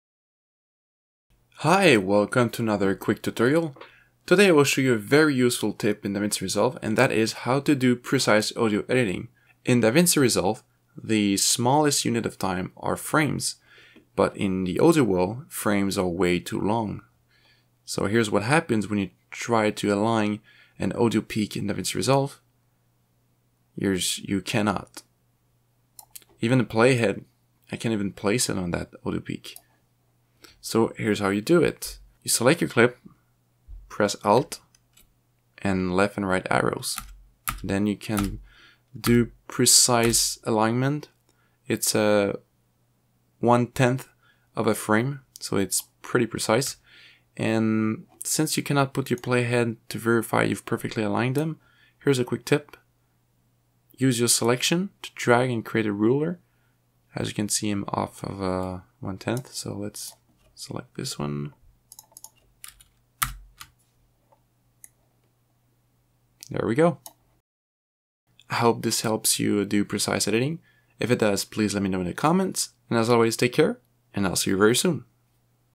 How to - Align audio between frames in DaVinci Resolve 12.5! (Subframe Audio Slip/nudge)
Here's how to align/sync audio clips precisely in DaVinci Resolve by increments smaller than frames!

No more frustration!

Tools used:
-ZOOM H4n
-OBS
-Audacity
-DaVinci Resolve 12.5
-VLC
-GIMP
-DaVinci Resolve 12.5 Reference Manual 2017 February, page 206
-SumatraPDF